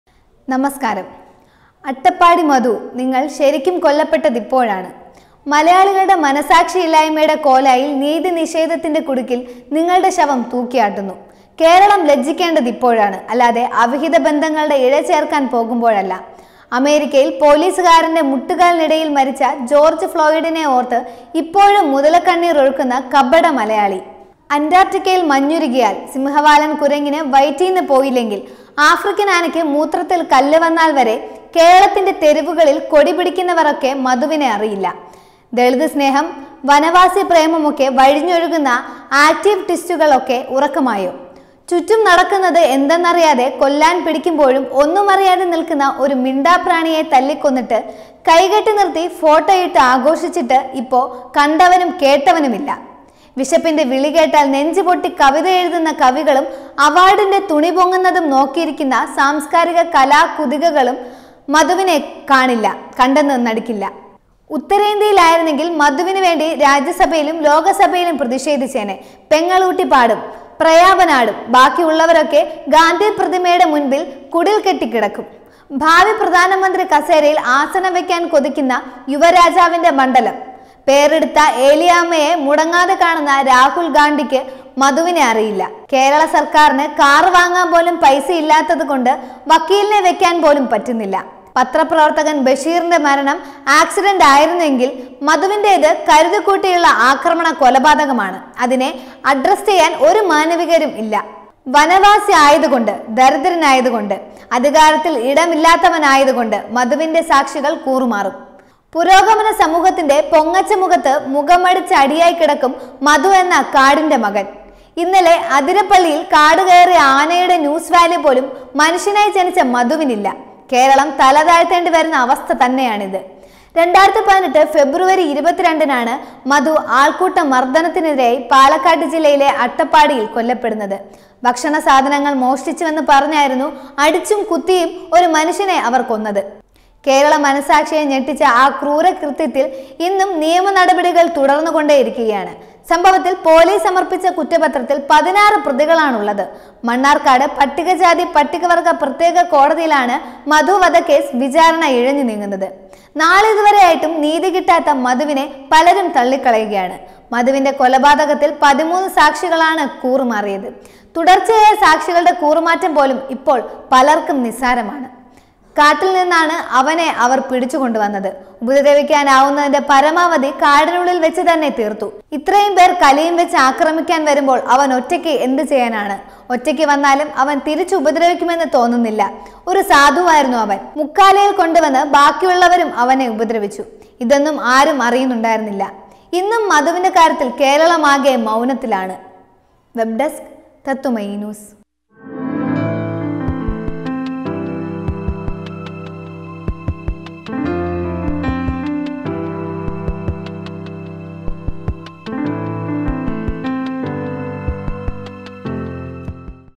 അട്ടപ്പാടി മധു, നിങ്ങൾ ശരിക്കും കൊല്ലപ്പെട്ടത് ഇപ്പോഴാണ്... | MADHU
ഉത്തരേന്ത്യയിൽ ആയിരുന്നെങ്കിൽ പെങ്ങളൂട്ടി പാടും, പ്രയാപൻ ആടും സാംസ്കാരിക നായകൾ കുരയ്ക്കും | TN PRATHAPAN